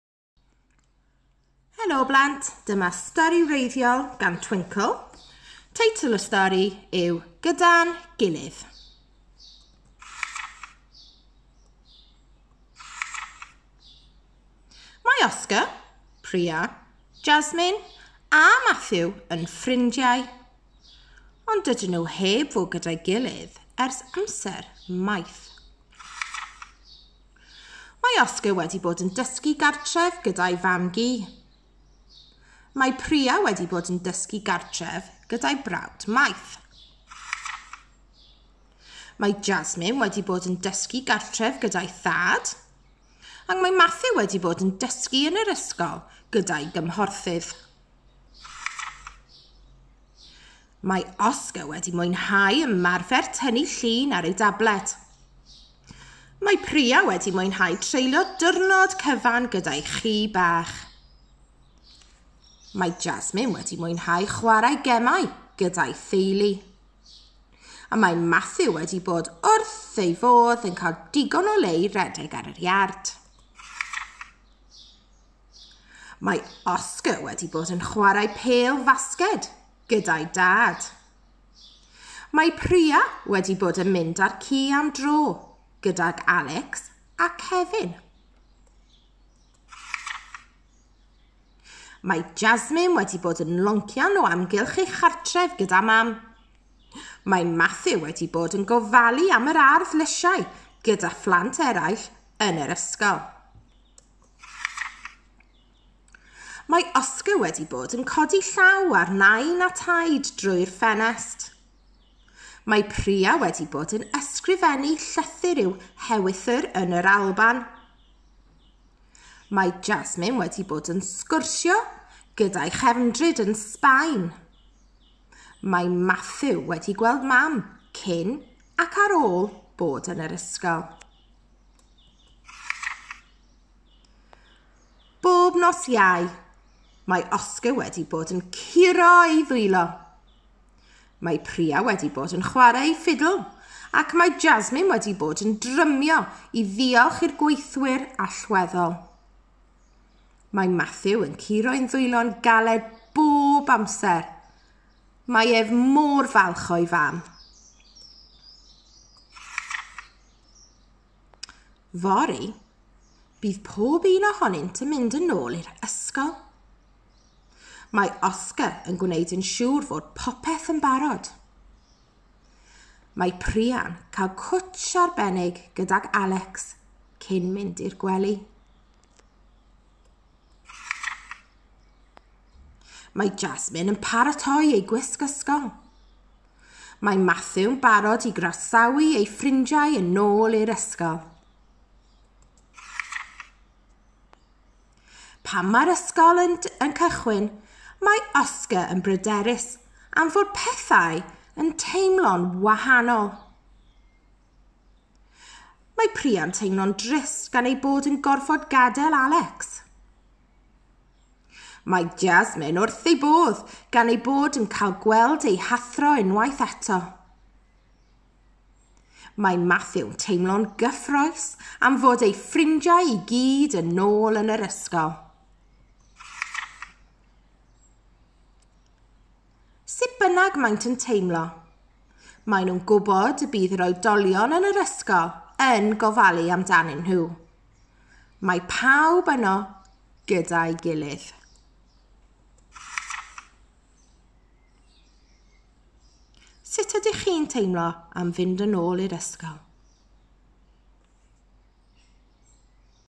Gyda'n Gilydd.
Stori wreiddiol gan Twinkl.

Mae Pryia, Oscar, Jasmin a Mathew yn paratoi i fynd yn ôl i'r ysgol.

Mae bob un ohonynt yn teimlo'n wahanol am hyn ond maent yn gwybod y byddant gyda'i gilydd.

Sut ydych chi'n teimlo am fynd yn ôl i'r ysgol?